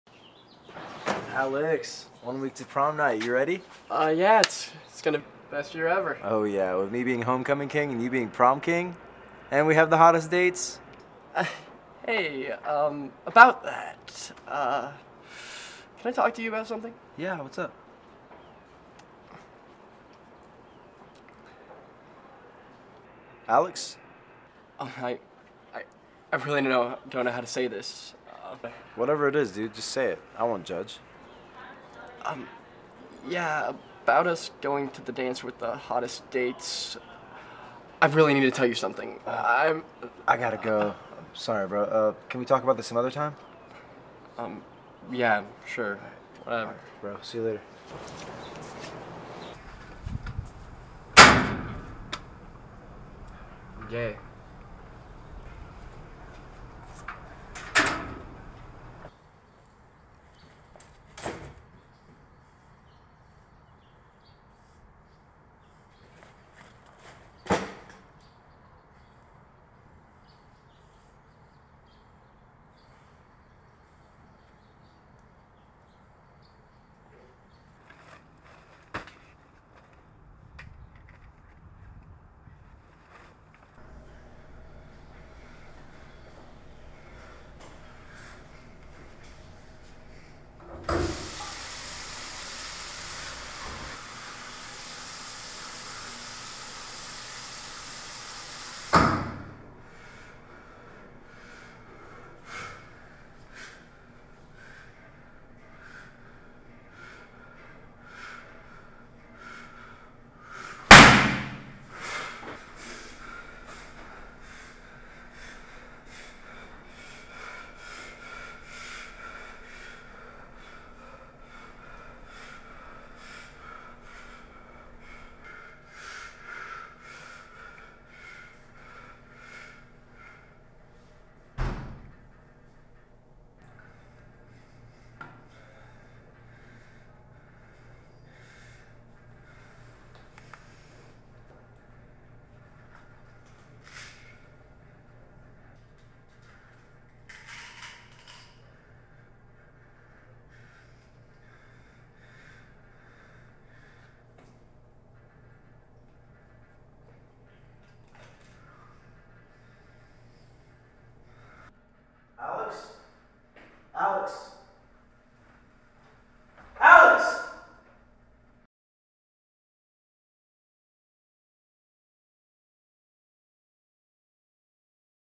What If (2017) | College Short Film Project | Short Film Production | Azcunaga's Production
Film Project I produced with Director: Anthony Rizo.

What If, tells the story of Alex (pink shirt), a high school senior who struggles to keep a secret and wants to come out clean to his best friend before prom.

This was my production with the Director Anthony Rizo for a college project back in 2017.

Unfortunately our lead actor couldn't continue working with us since your schedule at the navy and out xidnt work out.

It originally took place in a farm, where Alex tries and hangs himself. This was the end of the original script, the one we started shooting with since the location is a strawberry farm and that time was the most convenient for the noise.

Since we didn't have any other shooting but the end and without context about the story, we had no other choice but to reshoot everything.

This was the end result in which, I, believe this short movie speaks for itself.